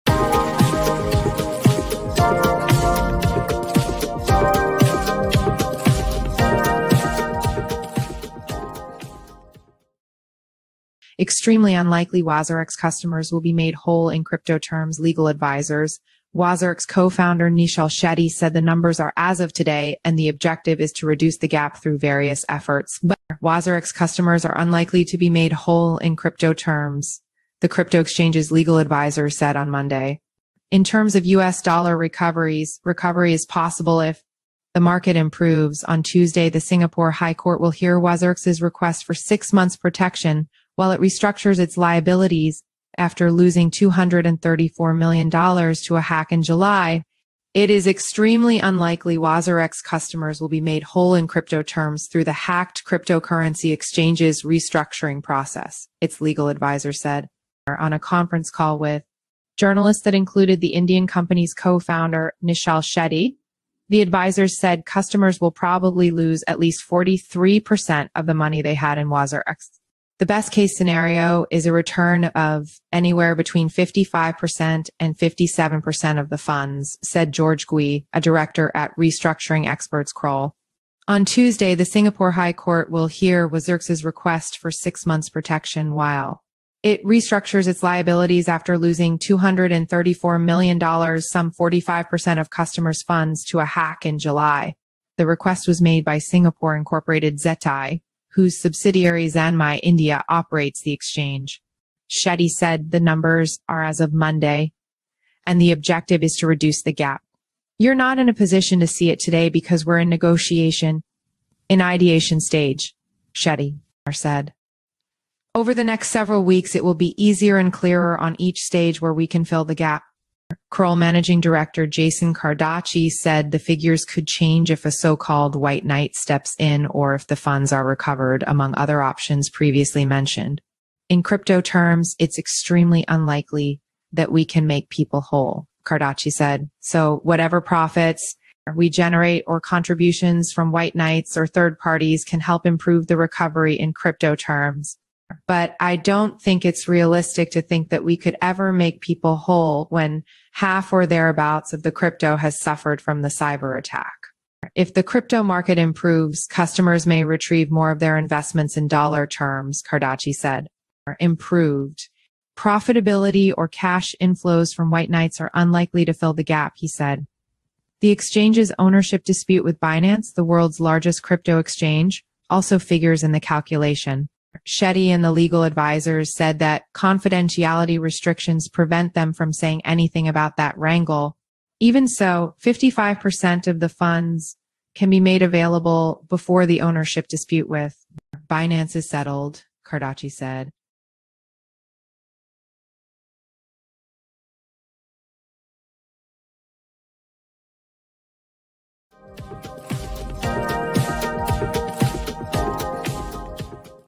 Extremely Unlikely WazirX Customers Will Be Made Whole in Crypto Terms: Legal Advisers
WazirX co-founder Nischal Shetty said the numbers are as of today and the objective is to reduce the gap through various efforts.
WazirX customers are unlikely to be made whole in crypto terms, the crypto exchange's legal advisers said on Monday.
In terms of U.S. dollar recoveries, recovery is possible if the market improves.
On Tuesday, the Singapore High Court will hear WazirX's request for six months' protection while it restructures its liabilities after losing $234 million to a hack in July.
It is extremely unlikely WazirX customers will be made whole in crypto terms through the hacked cryptocurrency exchange's restructuring process, its legal advisers said.
On a conference call with journalists that included the Indian company's co-founder Nischal Shetty, the advisers said customers will probably lose at least 43% of the money they had in WazirX. The best case scenario is a return of anywhere between 55% and 57% of the funds, said George Gwee, a director at restructuring experts Kroll.
On Tuesday, the Singapore High Court will hear WazirX's request for six months' protection while it restructures its liabilities after losing $234 million, some 45% of customers' funds, to a hack in July. The request was made by Singapore-incorporated Zettai, whose subsidiary Zanmai India operates the exchange.
Shetty said the numbers are as of Monday, and the objective is to reduce the gap.
"You're not in a position to see it today because we're in negotiation, in ideation stage," Shetty said. "Over the next several weeks, it will be easier and clearer on each stage where we can fill the gap."
Kroll managing director Jason Kardachi said the figures could change if a so-called white knight steps in or if the funds are recovered, among other options previously mentioned.
"In crypto terms, it's extremely unlikely that we can make people whole," Kardachi said. "So, whatever profits we generate or contributions from white knights or third parties can help improve the recovery in crypto terms. But I don't think it's realistic to think that we could ever make people whole when half or thereabouts of the crypto has suffered from the cyber attack."
If the crypto market improves, customers may retrieve more of their investments in dollar terms, Kardachi said. Improved profitability or cash inflows from white knights are unlikely to fill the gap, he said.
The exchange's ownership dispute with Binance, the world's largest crypto exchange, also figures in the calculation. Shetty and the legal advisers said that confidentiality restrictions prevent them from saying anything about that wrangle.
Even so, "55% of the funds can be made available before the ownership dispute with Binance is settled," Kardachi said.